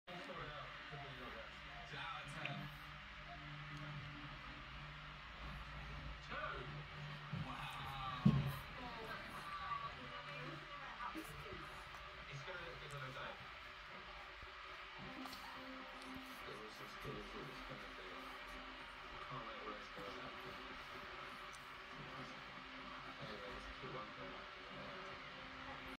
NEWCASTLE UNITED NEW SUMMER SIGNING £100,000,000 BEST EVER SIGNING
Luca has just signed for newcastle bless him best ever SIGNING